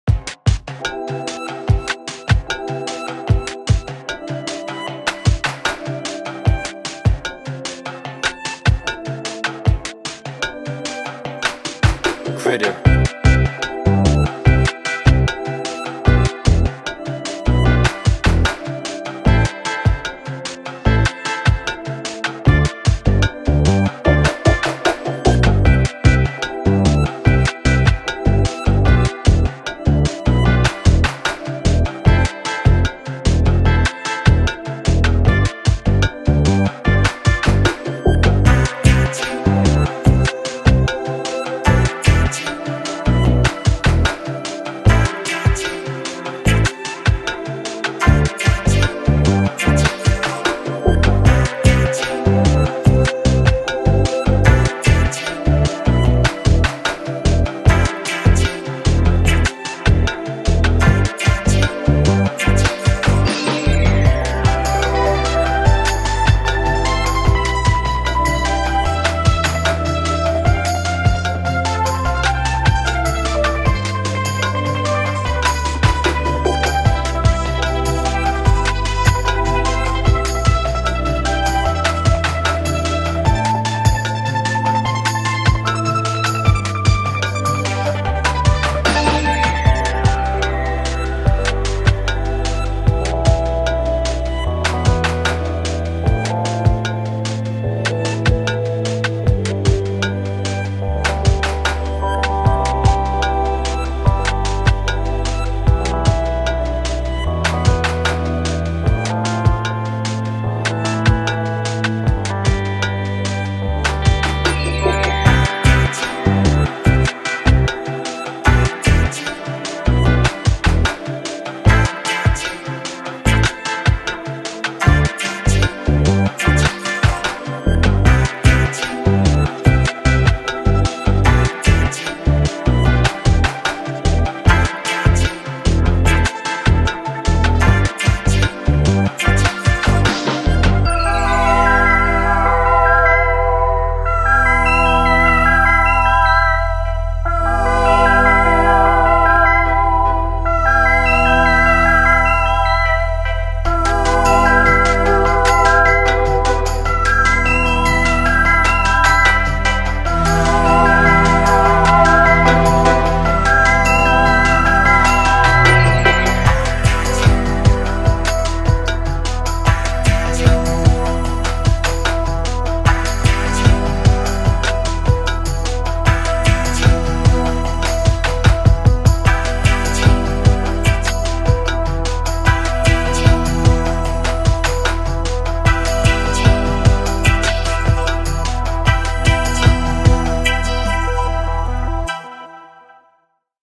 got u
chill idm breakbeats. i need to get some sleep lol. u-he diva is amazing! sounds sooo good im getting into analog synths now. it is good to sit at trees and look at clouds. i wish i had a tree and a clear sky. maybe one day.